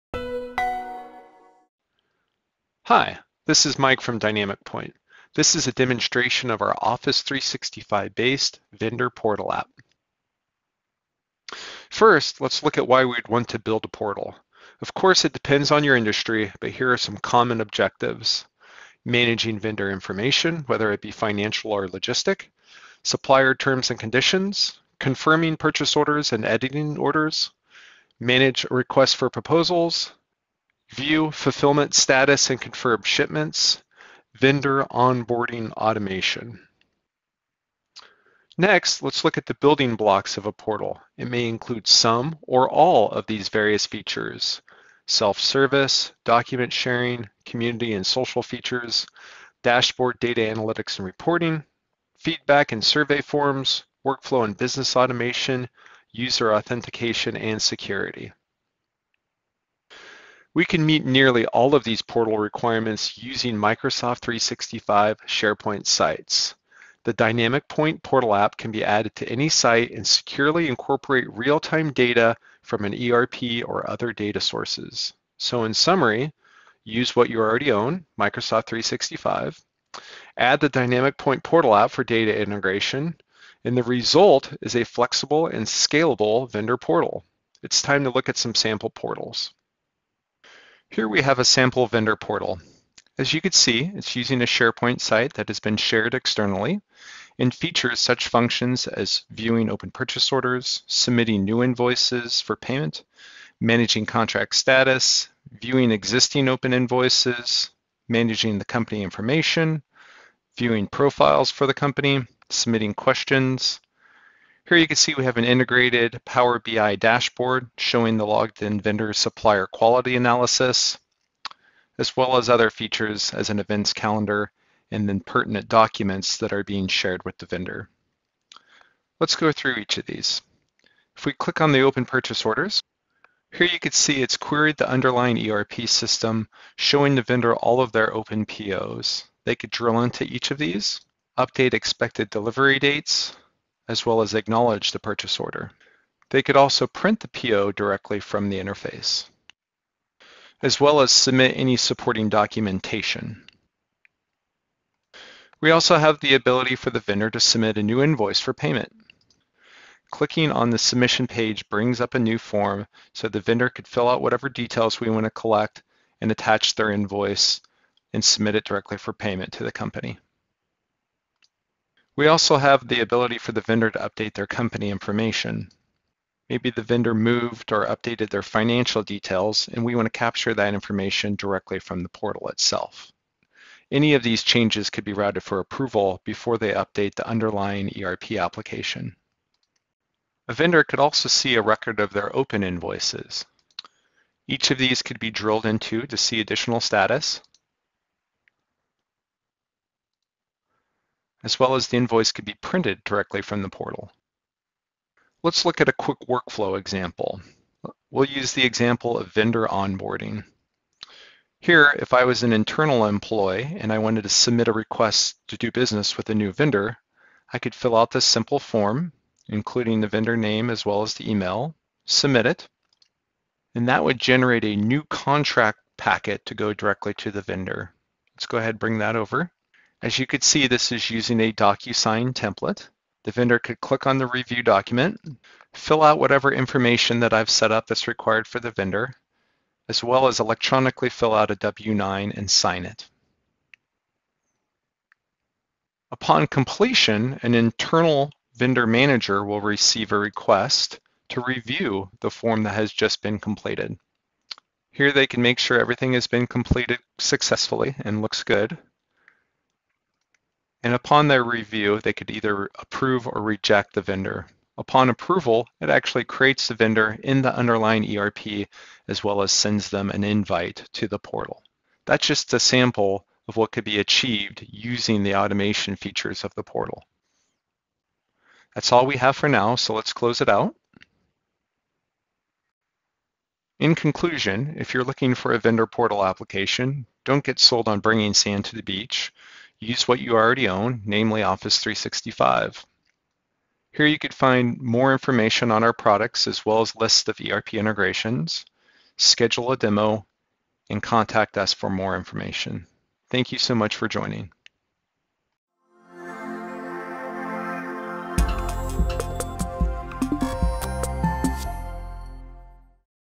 Quick Vendor Portal Demo | DynamicPoint, Inc.
DynamicPoint’s SharePoint Vendor Portal is built to integrate with your ERP and CRM system delivering a flexible, seamlessly integrated and highly secure extranet solution for 24/7 self-service access. Extend a view into your ERP system to ensure vendors can see the status of purchase orders, inventory, and invoices. Even allow them to update the status of existing PO's...

To learn more about our Customer, Vendor & Employee Portals App visit our product